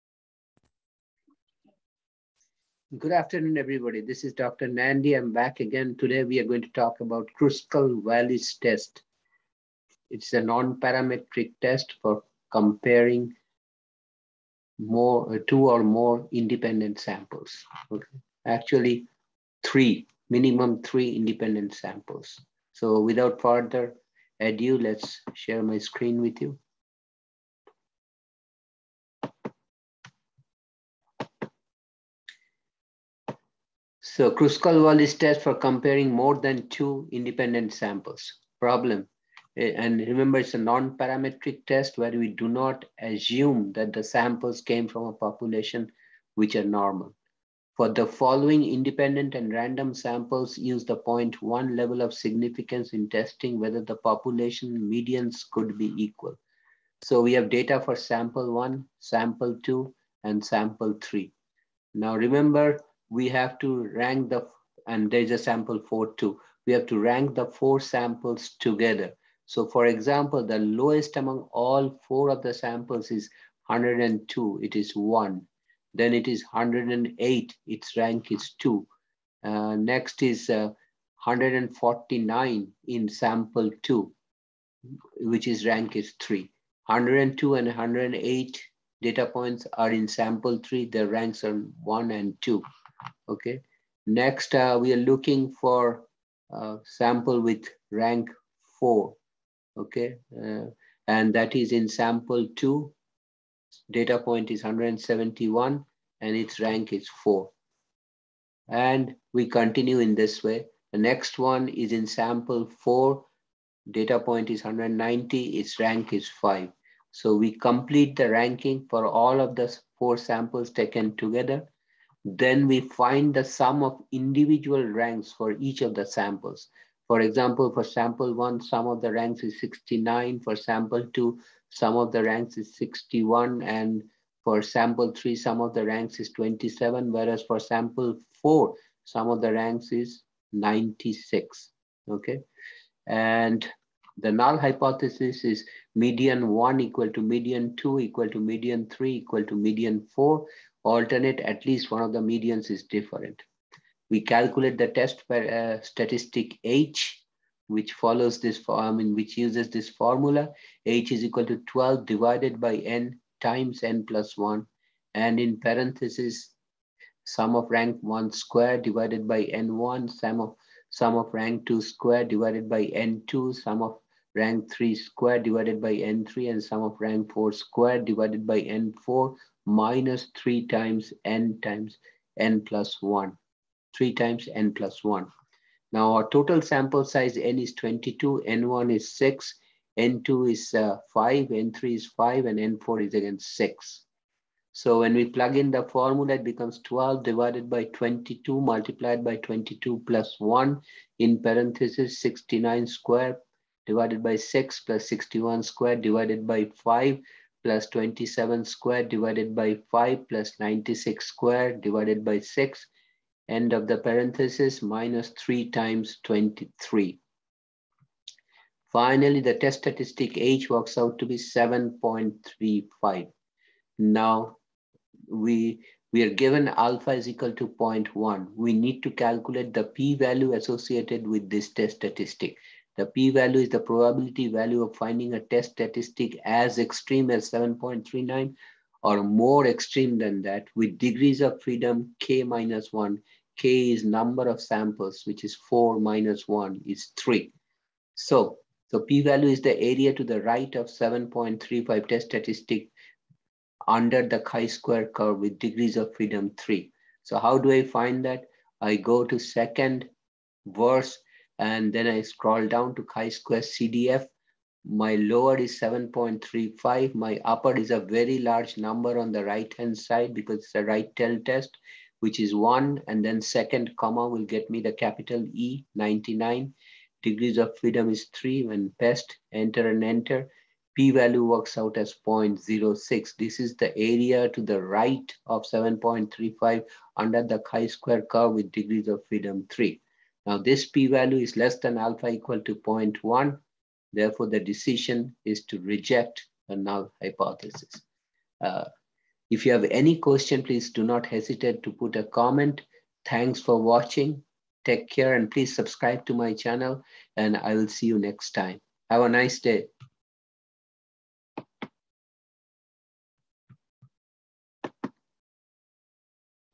HOW TO CONDUCT KRUSKAL-WALLIS TEST FOR INDEPENDENT SAMPLES
WORKED-OUT PROBLEM WITH TI 84